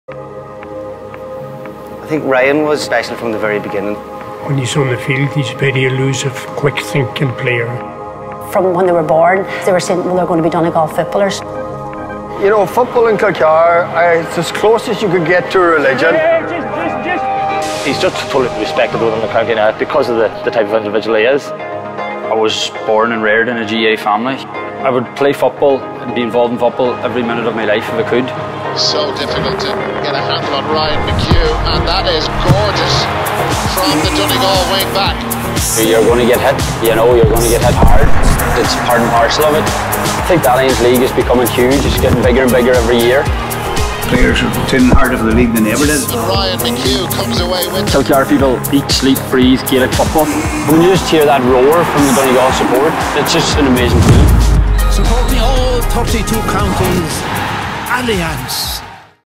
My Tribe: Ryan McHugh - Hero
Community, family and passion. Watch Ryan McHugh's journey in the  Allianz Leagues!